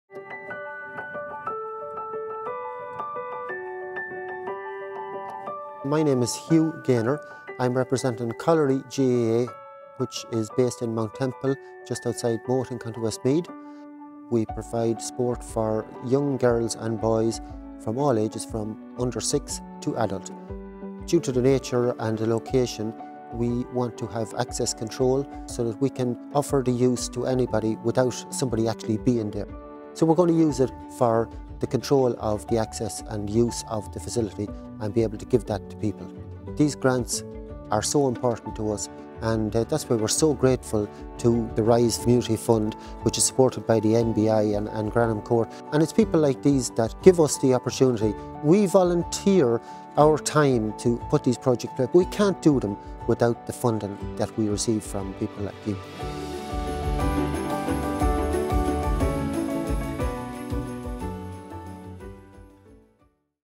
Caulry GAA in County Westmeath receives €1000 grant from the Rise Global Foundation
Rise Global Foundation Testimonial from Caulry GAA, Westmeath